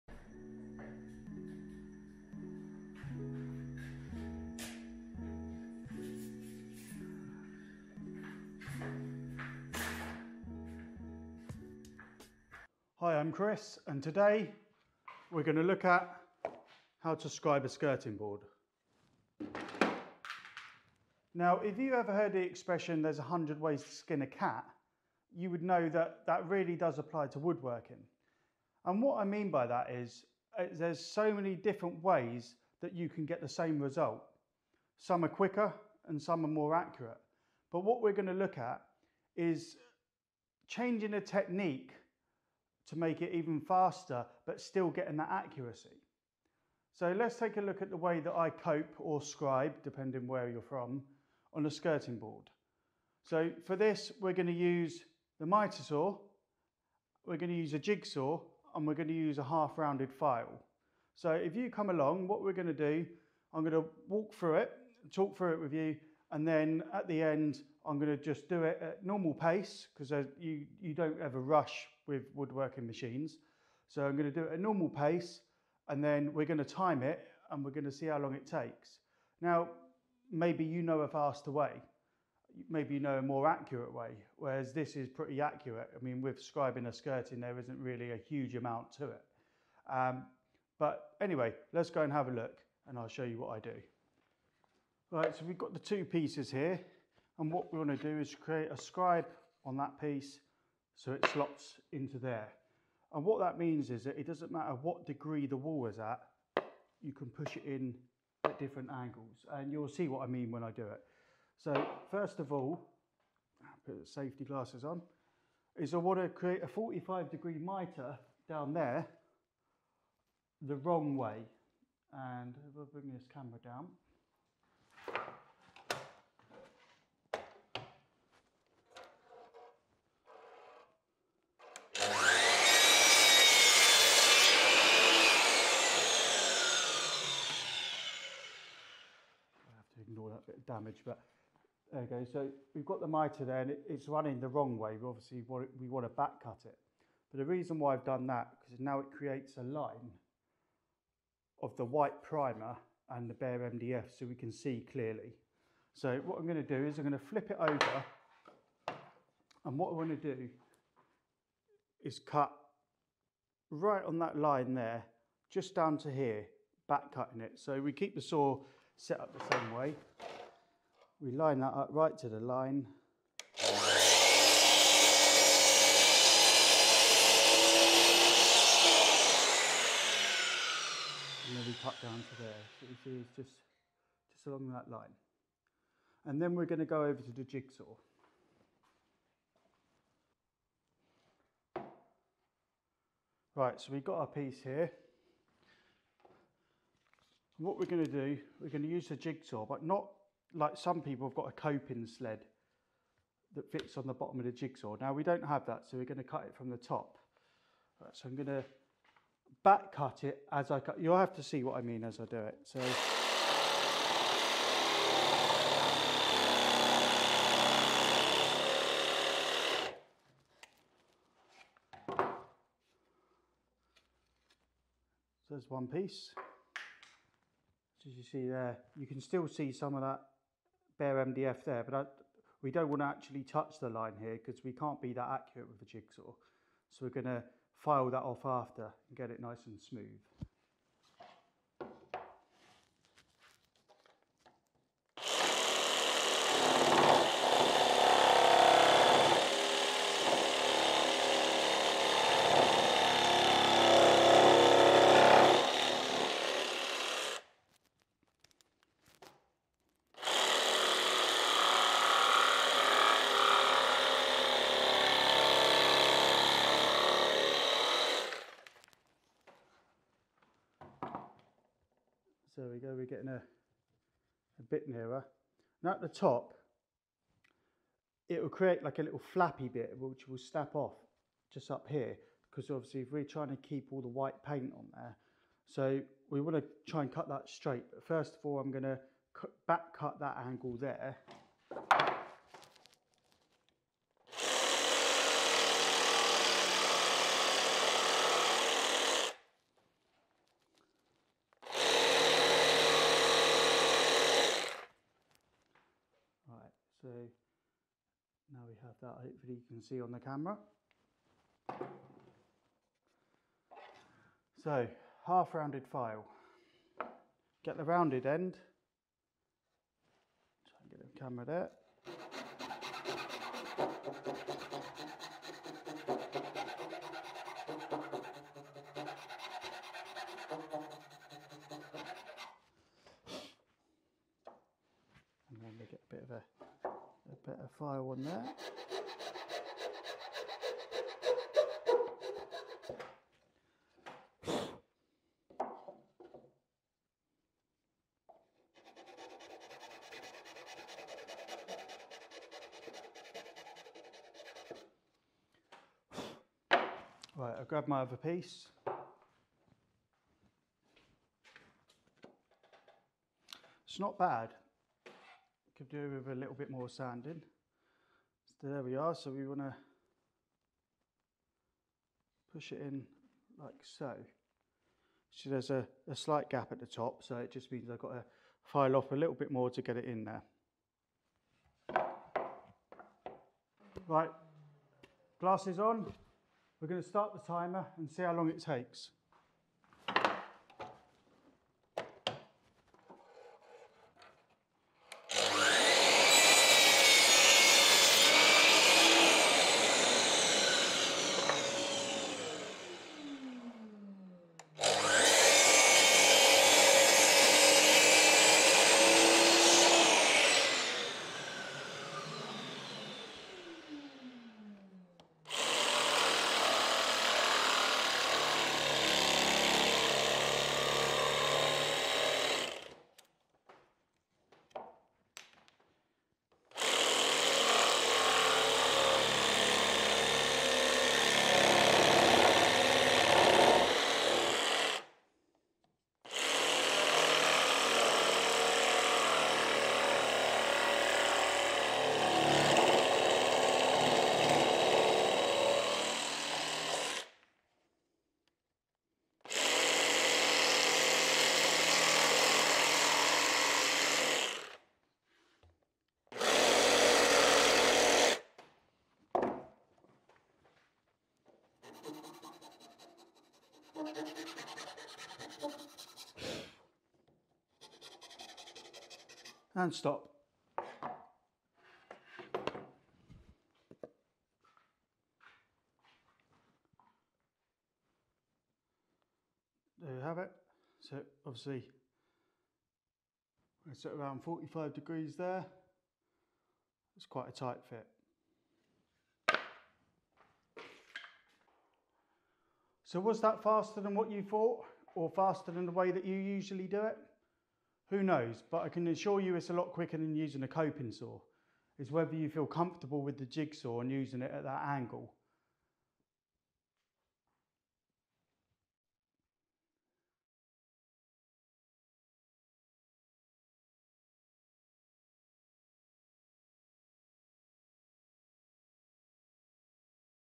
Is this the FASTEST way to scribe a skirting board??
A how to guide on How to scribe a skirting board in 1 minute 14 seconds. Coping / scribing skirting / base boards can be done in a variety of ways. This is the way that I use. Maybe you can scribe a skirting board more efficiently? If so, I would love to hear about it in the comments. What do you think is the fastest way to scribe a skirting board?

My saw Dewalt saw stand

Smaller Dewalt saw stand

My Dewalt mitre saw